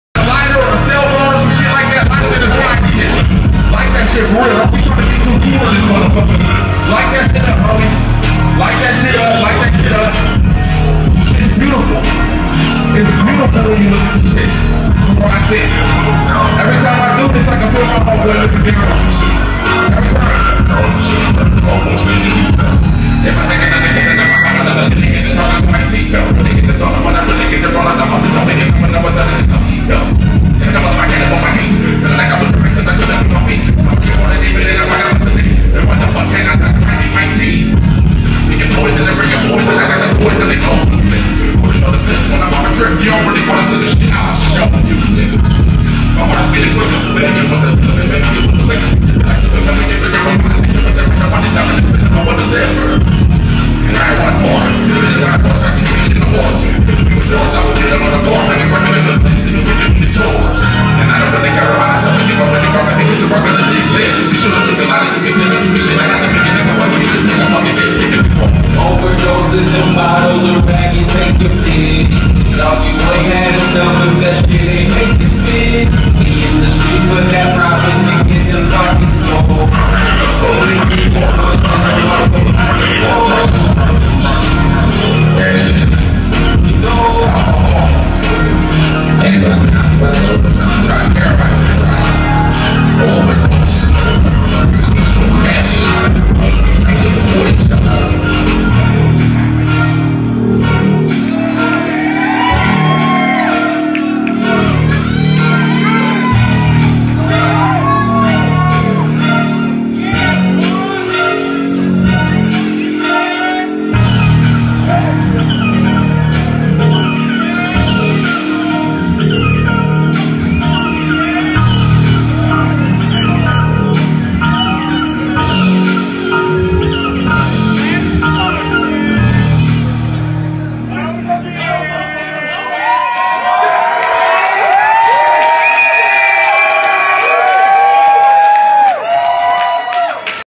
twisted insane
twisted insane centerstage bar and grill 3/12/2014 kokomo indiana da mafia 6